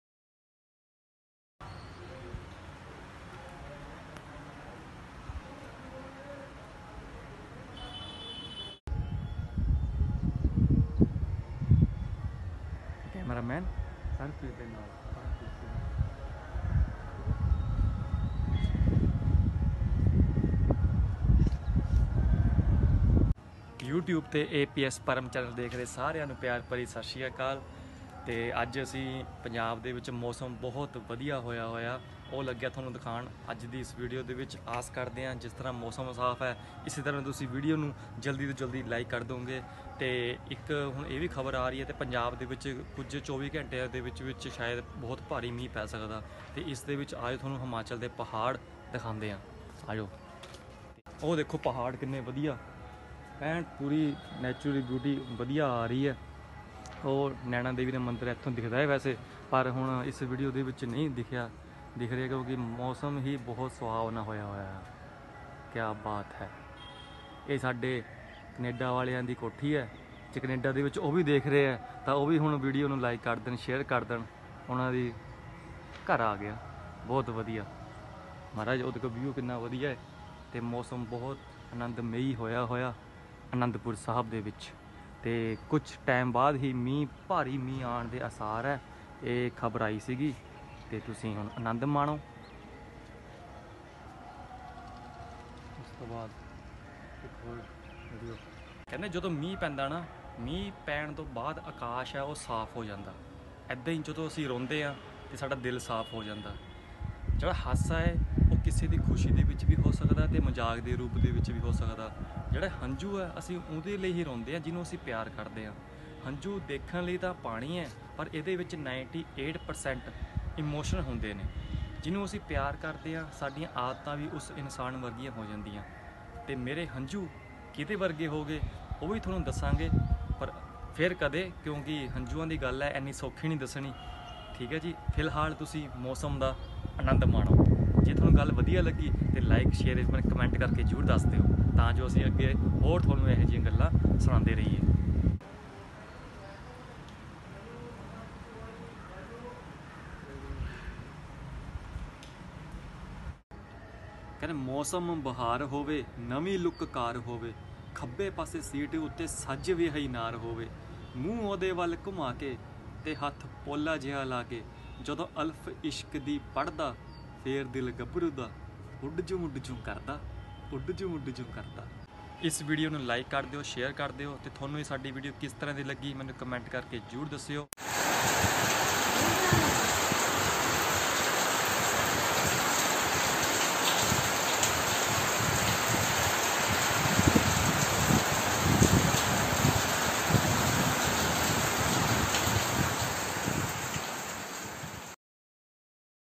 Beautiful weather*Shri Anandpur Sahib*Punjab*
You are watching Aps Param channel.
In this video, you will watch beautiful weather at Shri Anandpur Sahib(PUNJAB). This video only for fun and to see the naturally weather in Shri Anandpur Sahib.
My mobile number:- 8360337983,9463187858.